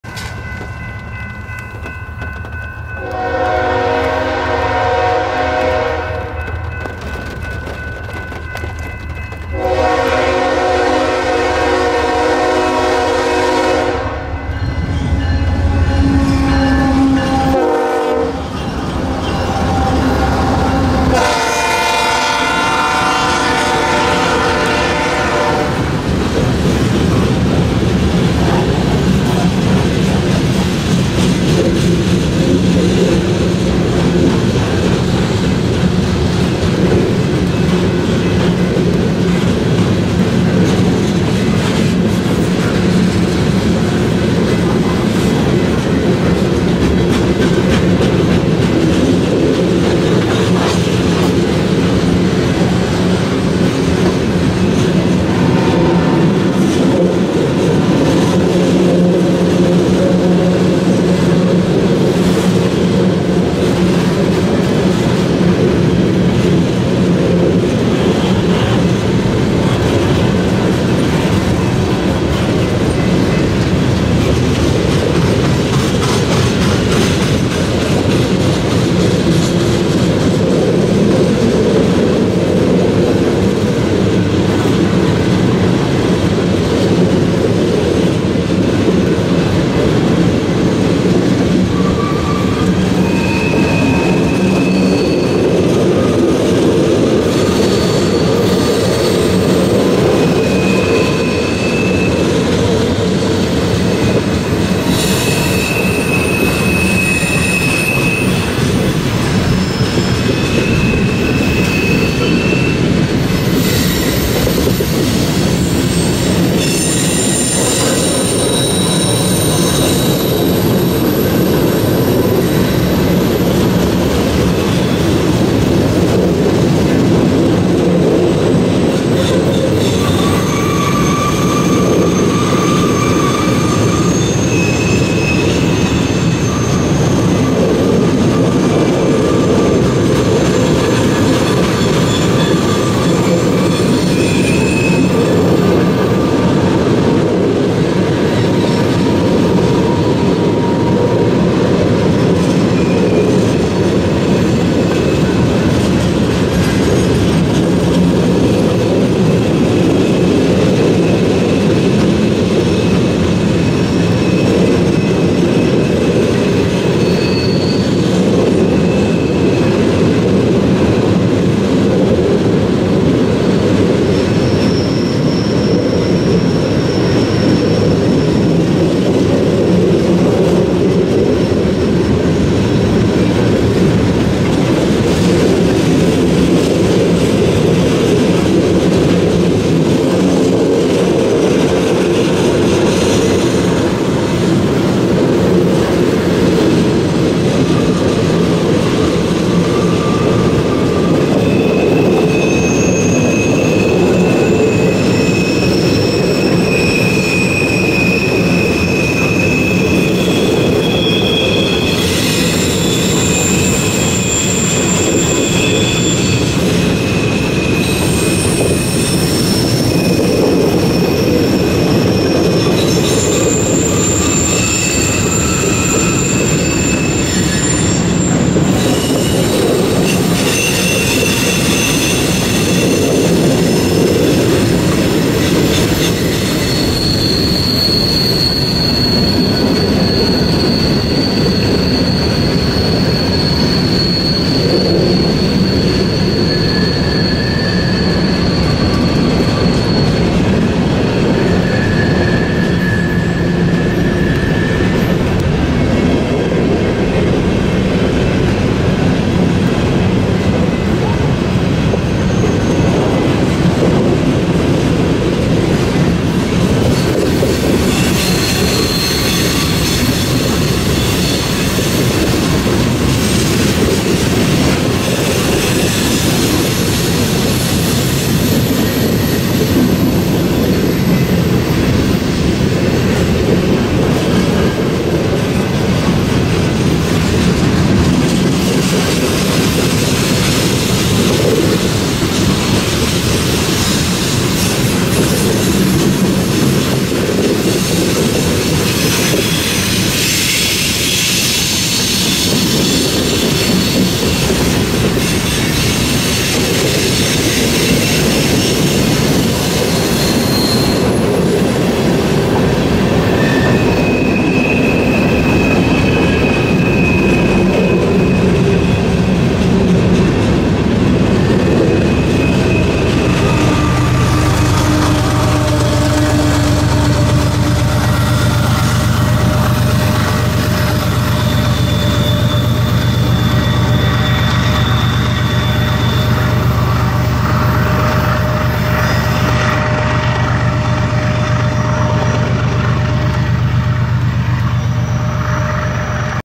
BNSF Freight On a Soggy Day, Colona, IL 3/8/24
A train in the rain. BNSF 6834,  8271,  & 5262 lead H-NTWKCK on the Barstow sub. DPU is BNSF 6203.